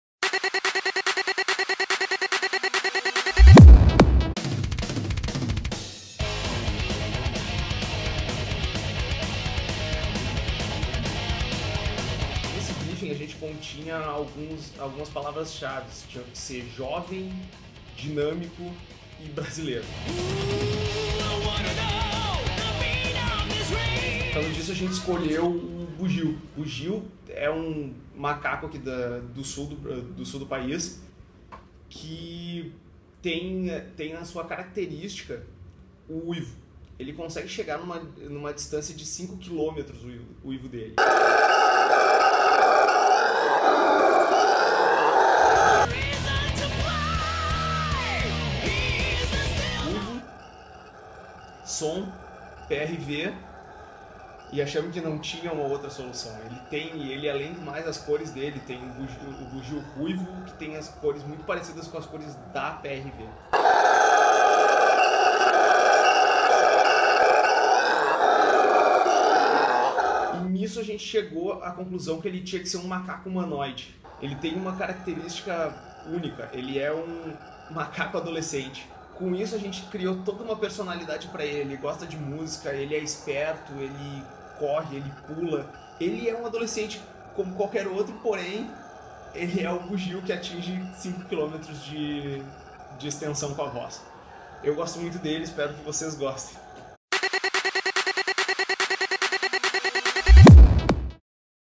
Gio – “The Bugio”, is the new PRV Audio mascot
Gio - The Bugio, will begin to accompany PRV Audio in all its events. “Our teen is leaving home. It’s time to leave the south of Brazil and start following PRV in its global actions, spreading the concept of sound strength and quality, together with the PRV Audio family.